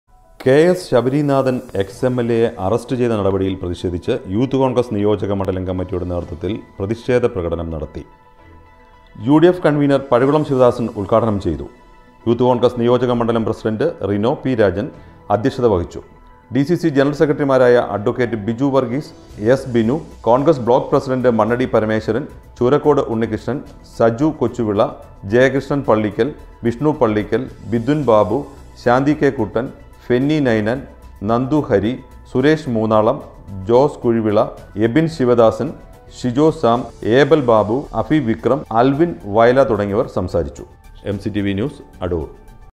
യൂത്ത് കോൺഗ്രസ് നിയോജക മണ്ഡലം കമ്മറ്റിയുടെ നേതൃത്വത്തിൽ പ്രതിഷേധ പ്രകടനം നടത്തി.ADOOR/ADOORNEWS
കെ.എസ് ശബരിനാഥൻ  Ex.എം.എൽ.എയെ അറസ്റ്റ് ചെയ്ത നടപടിയിൽ പ്രതിഷേധിച്ച് യൂത്ത് കോൺഗ്രസ് നിയോജക മണ്ഡലം കമ്മറ്റിയുടെ നേതൃത്വത്തിൽ പ്രതിഷേധ പ്രകടനം നടത്തി.യു.ഡി.എഫ് കൺവീനർ പഴകുളം ശിവദാസൻ ഉദ്ഘാടനം ചെയ്തു. യൂത്ത് കോൺഗ്രസ് നിയോജകമണ്ഡലം പ്രസിഡന്റ് റിനോ പി.രാജൻ അദ്ധ്യക്ഷത വഹിച്ചു. ഡി.സി.സി ജനറൽ സെക്രട്ടറിമാരായ അഡ്വ. ബിജു വർഗ്ഗീസ്, എസ്.ബിനു, കോൺഗ്രസ് ബ്ലോക്ക് പ്രസിഡന്റ് മണ്ണടി പരമേശ്വരൻ , ചൂരക്കോട് ഉണ്ണിക്കൃഷ്ണൻ , സജു കൊച്ചുവിള, ജയകൃഷ്ണൻ പള്ളിക്കൽ, വിഷ്ണു പള്ളിക്കൽ, ബിഥുൻ ബാബു, ശാന്തി കെ.കുട്ടൻ, ഫെന്നി നൈനാൻ , നന്ദു ഹരി, സുരേഷ് മൂന്നാളം, ജോസ് കുഴിവിള , എബിൻ ശിവദാസൻ , ഷിജോ സാം, ഏബൽ ബാബു, അഭി വിക്രം, ആൽവിൻ വയല തുടങ്ങിയവർ സംസാരിച്ചു...